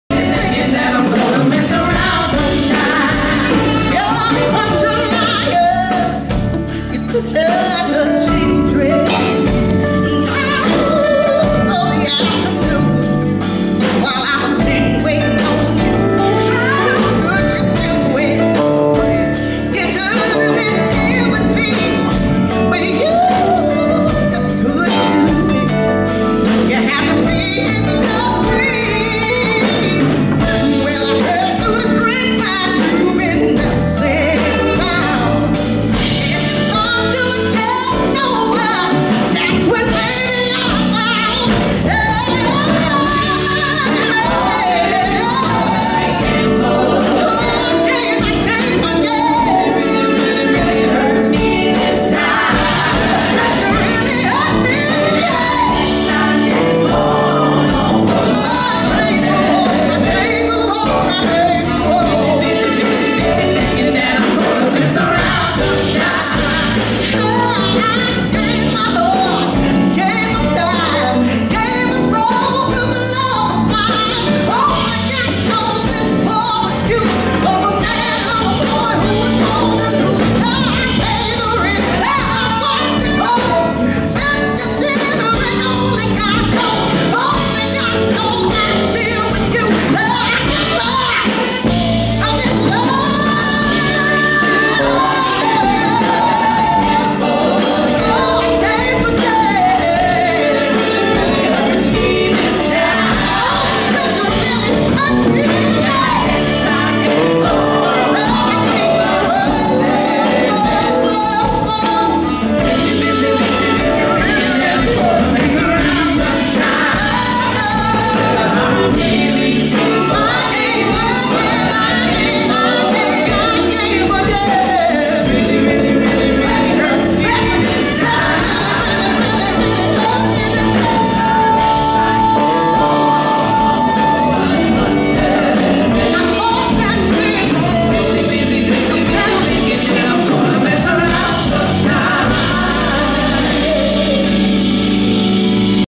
ARETHA FRANKLIN;TRIBUTE REHEARSAL 2018 -A TVC VIDEO
DETROIT'S MUSIC HALL WAS THE PLACE
THE SHOW WAS ASOME !!!
SO MUCH LOVE, FOR  OUR  "QUEEN'"